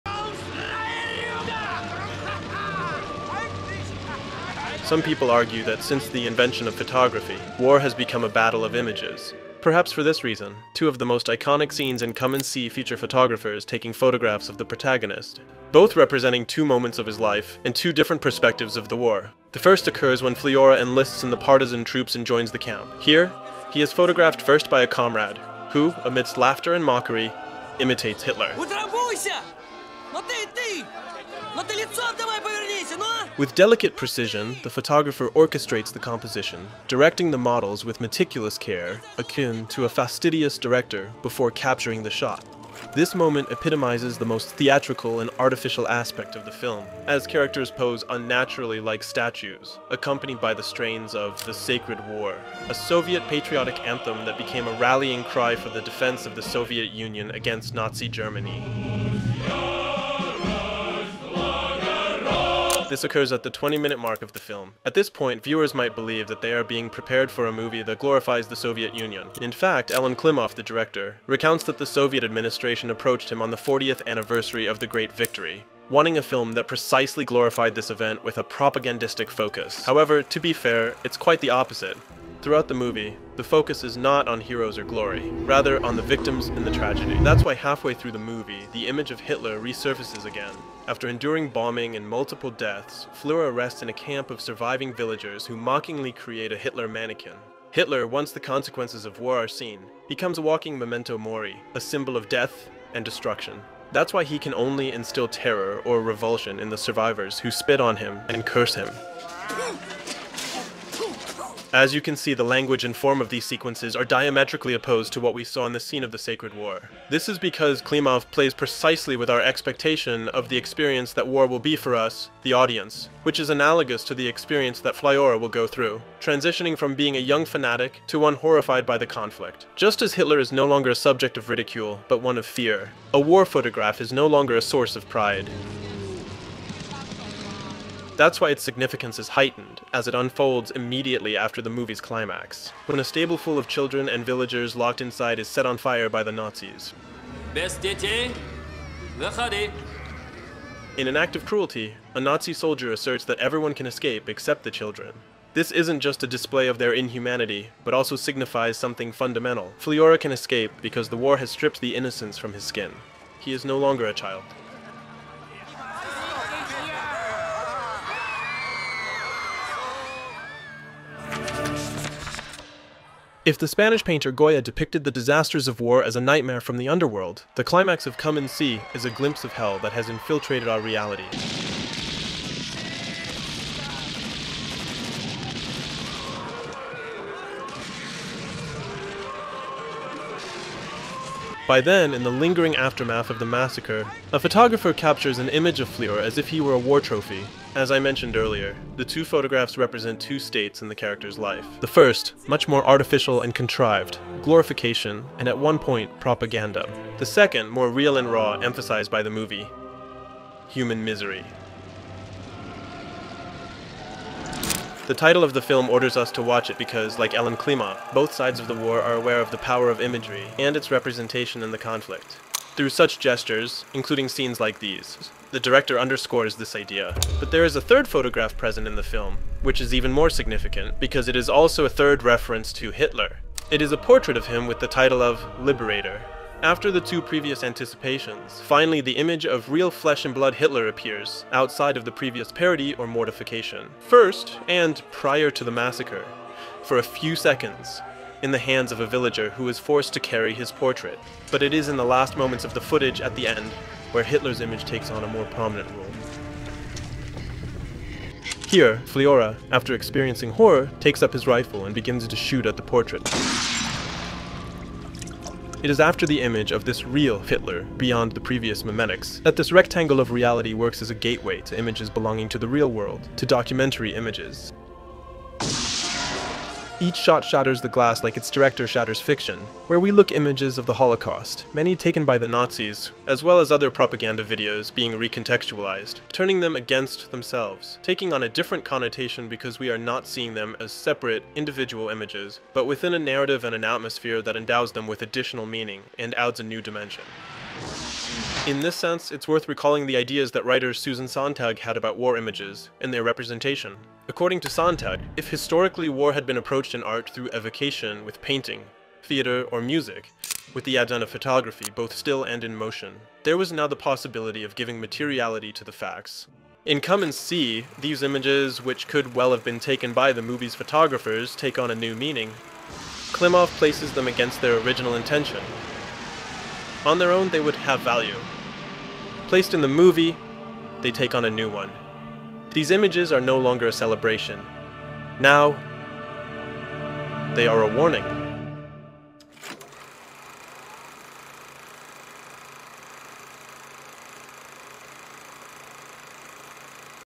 they used real footage on a war movie...
A film essay of Come And See and why is the best propaganda film ever made.

In this video, we delve into the realm of war cinema, exploring its intricate portrayal of the human condition amidst the chaos of conflict. Focusing particularly on Elem Klimov's masterpiece "Come and See," we unravel how this film transcends conventional war narratives to confront the propaganda machinery head-on. "Come and See" serves as a stark reminder of the harrowing realities of war, offering a visceral experience that exposes the manipulative tactics of wartime propaganda. Through compelling storytelling and haunting imagery, the film challenges viewers to question the glorification of war and confront the profound consequences it inflicts upon individuals and societies. Join us as we dissect the profound impact of "Come and See" and its enduring legacy in the realm of war cinema.